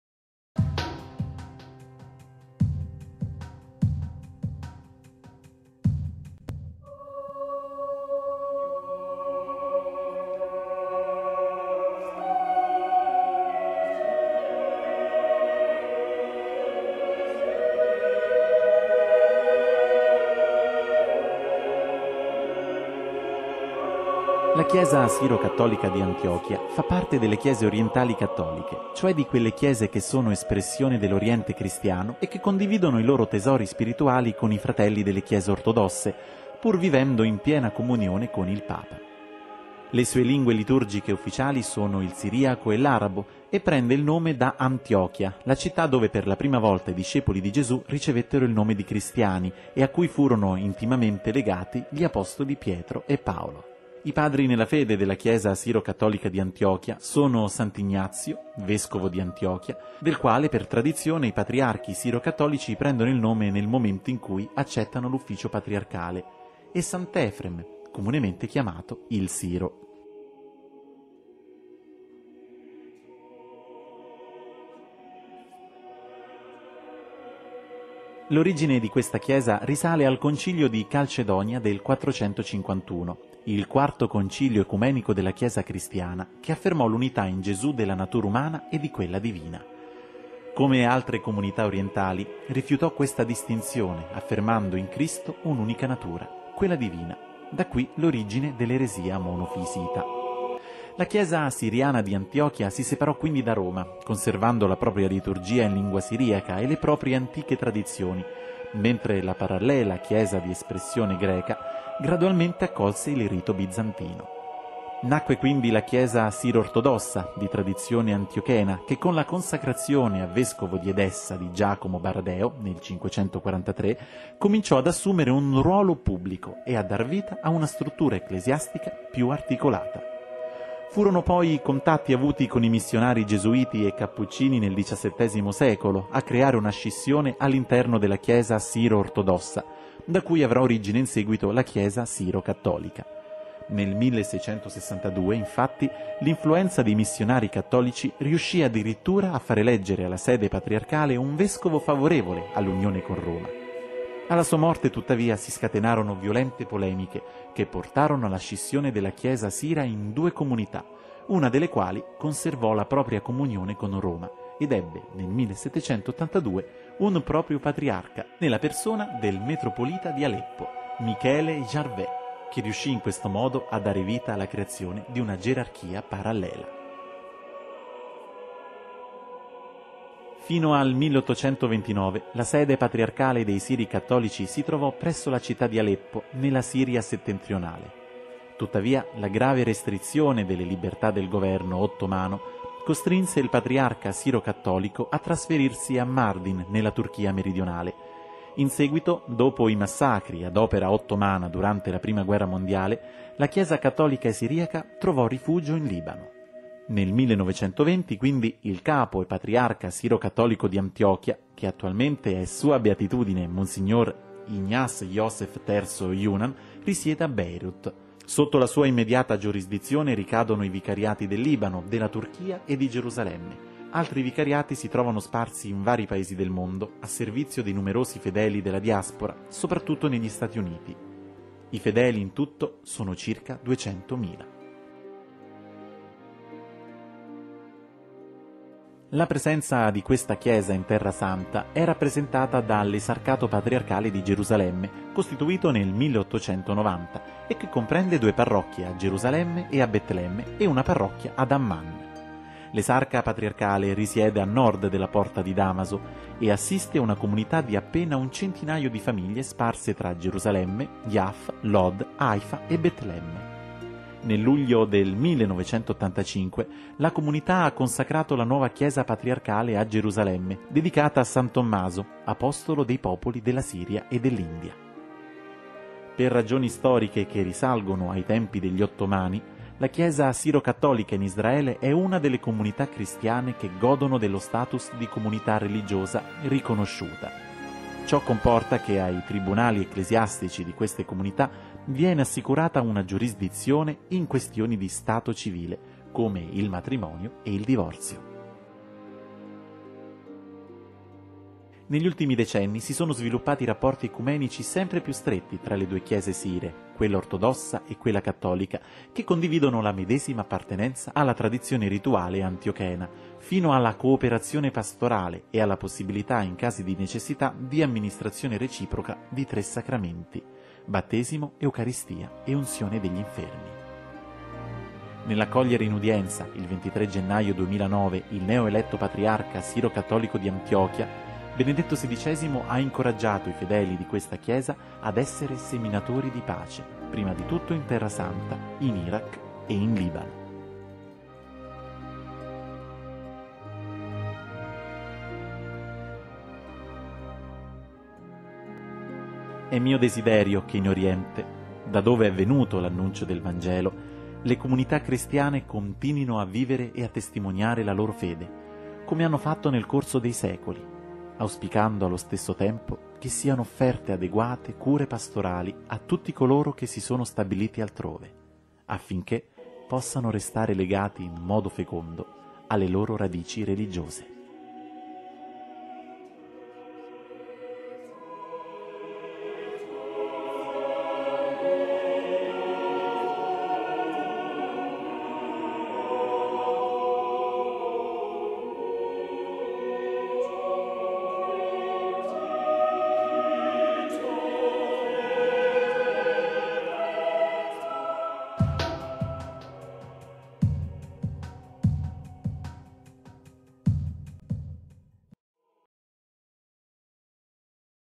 Chiesa siro-cattolica di Antiochia
La Chiesa siro-cattolica di Antiochia fa parte delle Chiese orientali cattoliche, cioè di quelle Chiese che sono espressione dell'Oriente cristiano e che condividono i loro tesori spirituali con i fratelli delle Chiese ortodosse, pur vivendo in piena comunione con il Papa.